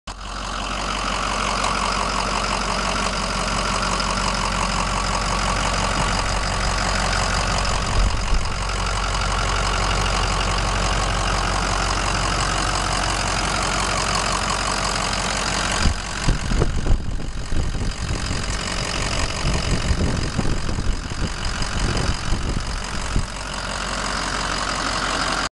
GovDeals: F-550 side loader refuse truck
F-550 side loader refuse truck sold by Raleigh, NC on GovDeals.com